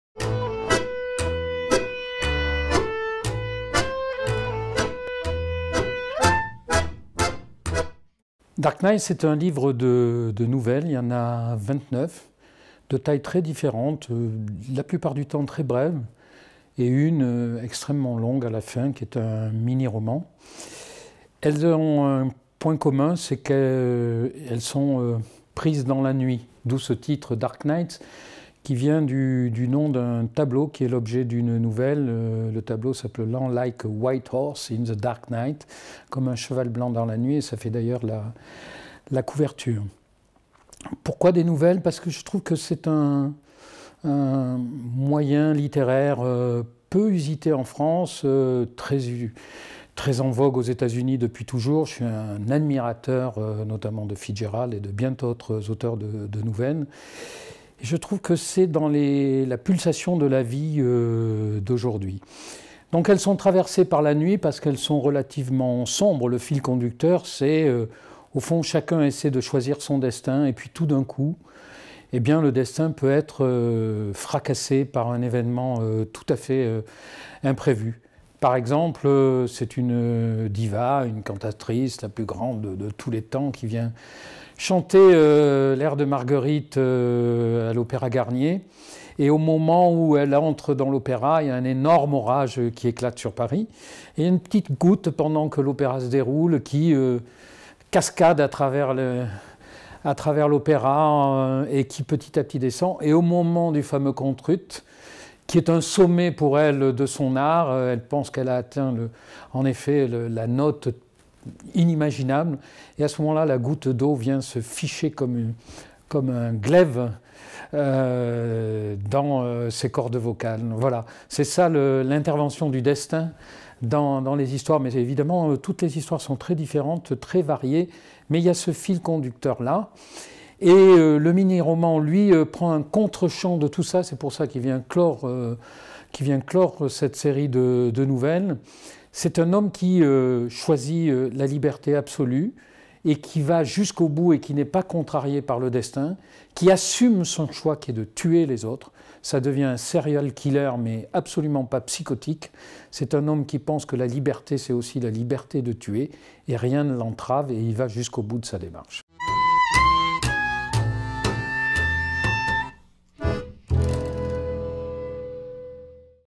Denis Jeambar - Dark nights, nouvelles nocturnes
A l'occasion du Salon du Livre de Paris 2014, Denis Jeambar vous présente son ouvrage "Dark nights, nouvelles nocturnes" aux éditions Calmann-Lévy.